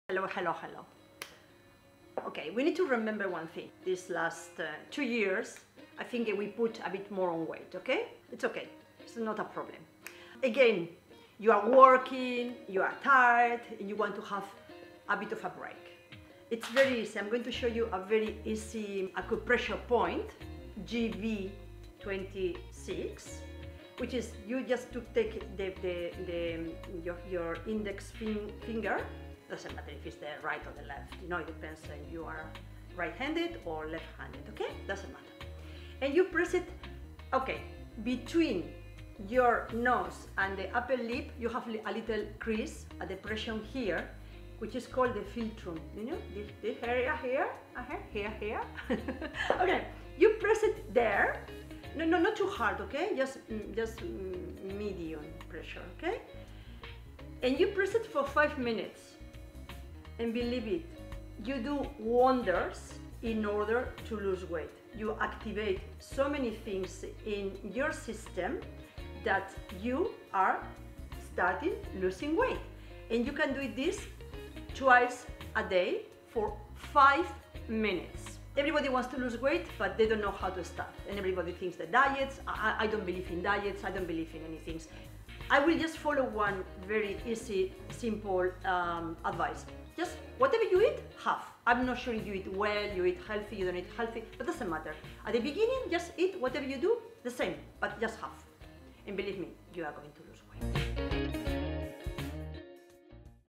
Want to lose weight? Very simple tip: acupressure is here for you!
Pressing the area between the nose and the mouth activates a pressure point that does wonders in order to lose weight.